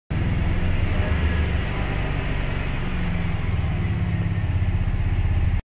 GovDeals: 2003 Dodge Ram 1500 ST Quad Cab Short Bed 4WD
2003 Dodge Ram 1500 ST Quad Cab Short Bed 4WD sold by Dept of General Services Office of Fleet & Asset Mngmt, CA on GovDeals.com